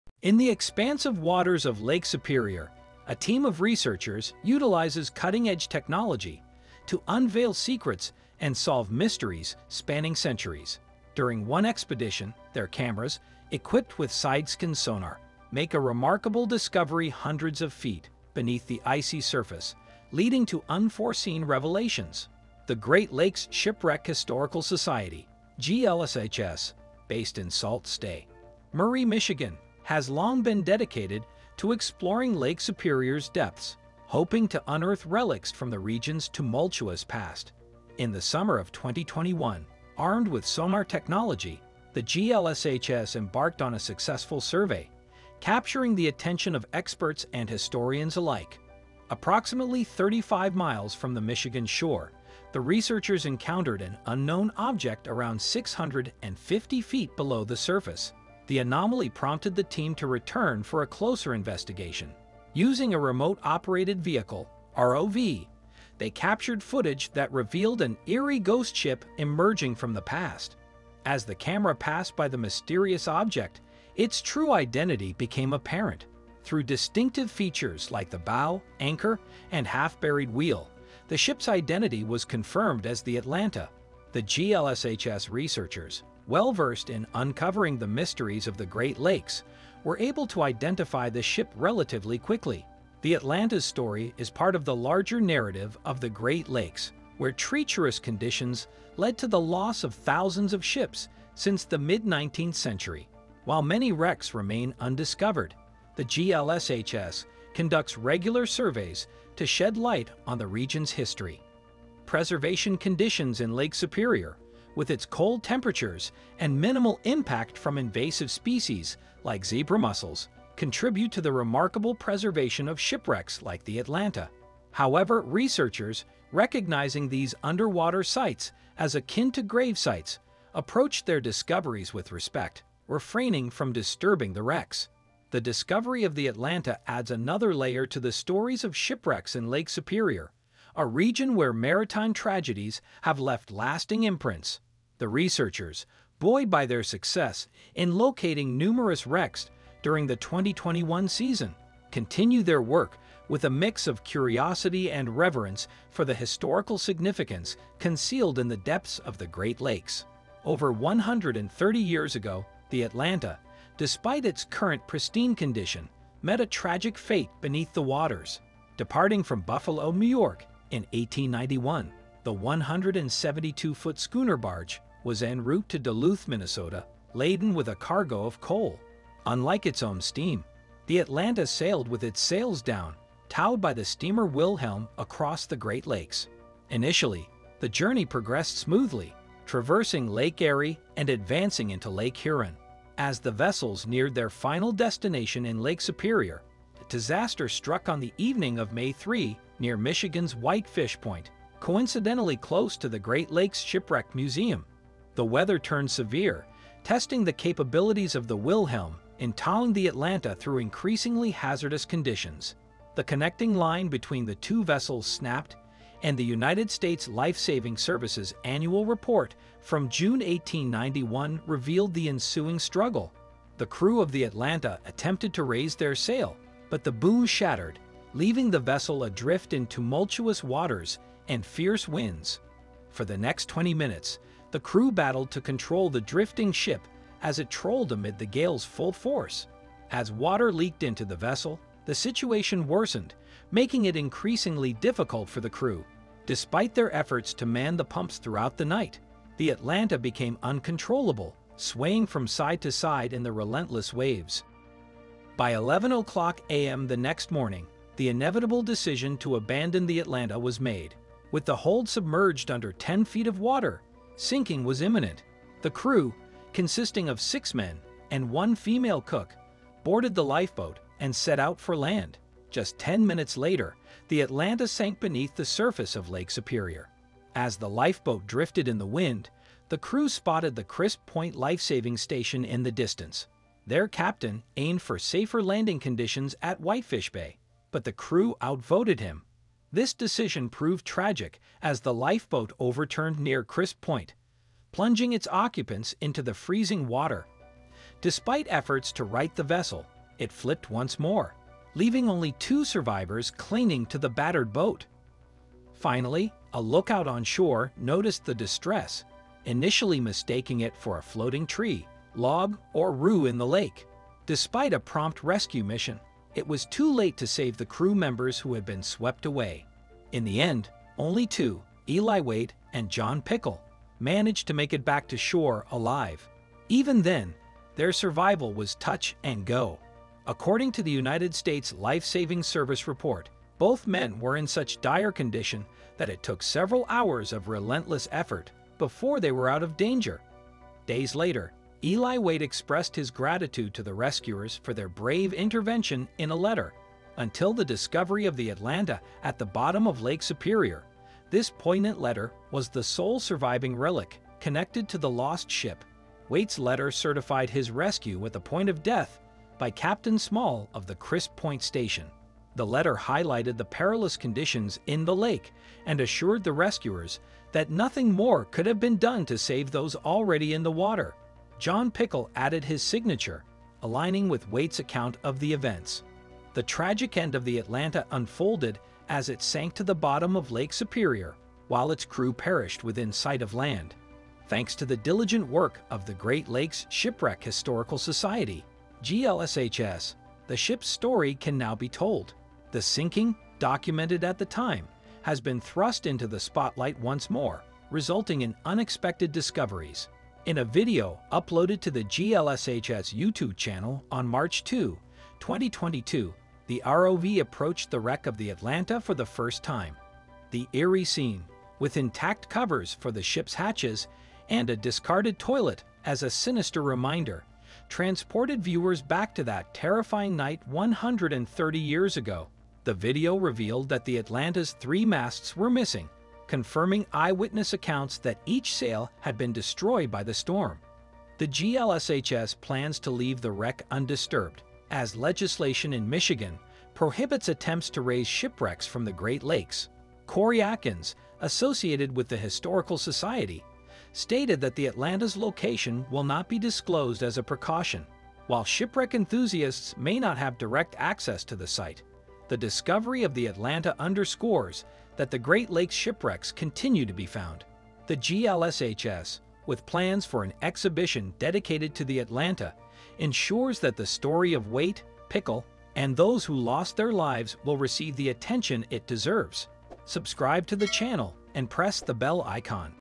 Cameras Unearth Unbelievable Lake Superior Secrets | Deep Dive Marvels
In the expansive reaches of Lake Superior, a team of researchers explores the icy depths, deploying cutting-edge technology to unveil age-old secrets and solve mysteries spanning centuries. However, their journey takes an unexpected turn when they chance upon a peculiar feature hundreds of feet beneath the surface, revealing more than they anticipated.

1. The videos have no negative impact on the original works.
4. We use only the audio component and tiny pieces of video footage, only if it's necessary.